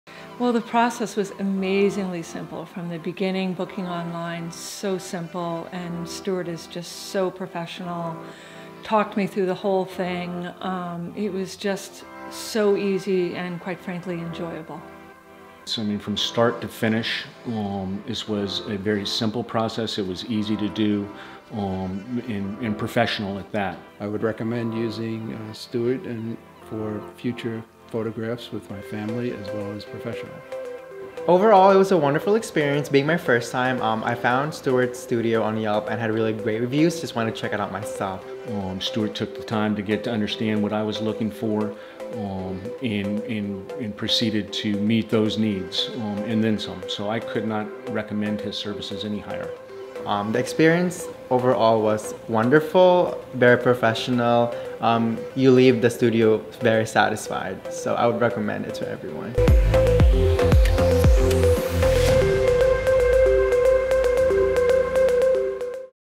Headshot Photography Client Testimonials for Stuart Beeby Photography
Our Studio Headshot Sessions are extremely popular and we have helped many Executives, Entrepreneurs, Speakers, and College Grads update their Online Professional Profiles with vibrant, natural-looking headshots.
In this video, we share the wonderful feedback our valued clients have given our studio.